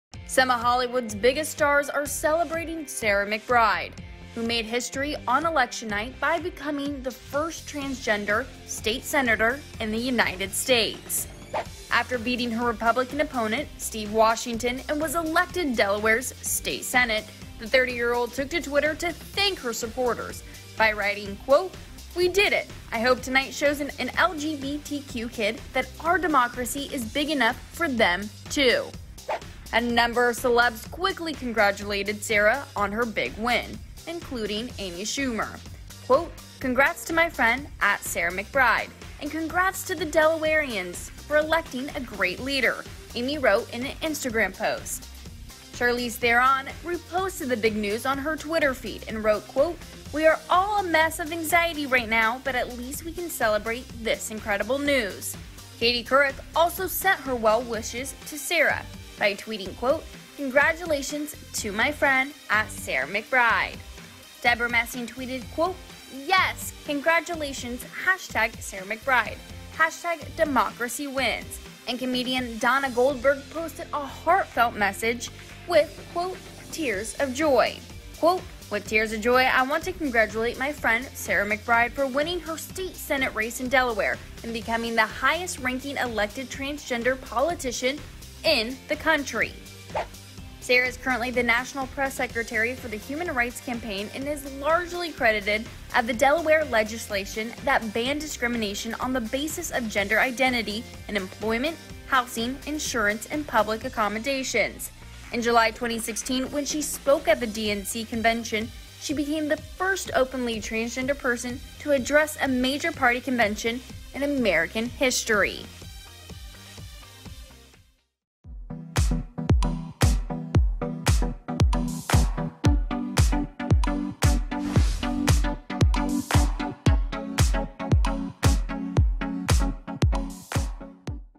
Sarah McBride Praised For Being First Transgender State Senator
Some of Hollywood's biggest stars are celebrating Sarah McBride! The 30-year-old made history on election night by becoming the first transgender state senator in the United States. After beating her republican opponent Steve Washington and was elected Delaware's state senate, she took to twitter to thank her supporters, writing, "We did it. I hope tonight shows an lgbtq kid that our democracy is big enough for them, too." A number of celebs quickly congratulated Sarah on her big win, including Amy Schumer and Charlize Theron.